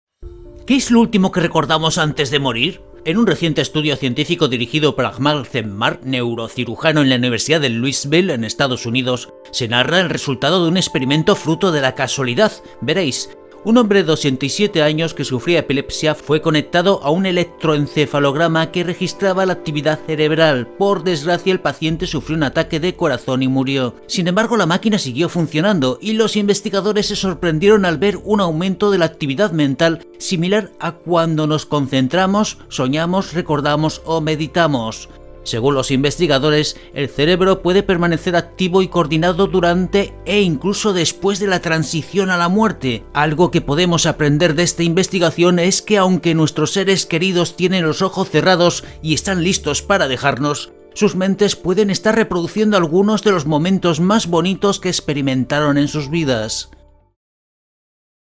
¿Qué es lo Último que Recordamos Antes de Morir? Hallazgo sobre el cerebro humano
En un reciente estudio científico dirigido por Ajmal Zemmar, neurocirujano en la Universidad de Louisville en EEUU. se narra el resultado de un experimento fruto de la casualidad. Veréis.
Un hombre de 87 años, que sufría de epilepsia, fue conectado a un electroencefalograma que registraba la actividad cerebral, por desgracia el paciente sufrió un ataque de corazón y murió, pero la máquina siguió funcionando. Los investigadores se sorprendieron al ver un aumento de la actividad mental similar a cuando nos concentramos, soñamos, recordamos o meditamos. Según los investigadores: "El cerebro puede permanecer activo y coordinado durante e incluso después de la transición a la muerte. Algo que podemos aprender de esta investigación es que, aunque nuestros seres queridos tienen los ojos cerrados y están listos para dejarnos, sus mentes pueden estar reproduciendo algunos de los momentos más bonitos que experimentaron en sus vidas".

EXOPLANETAS:
Ayuda a crear más y mejores vídeos: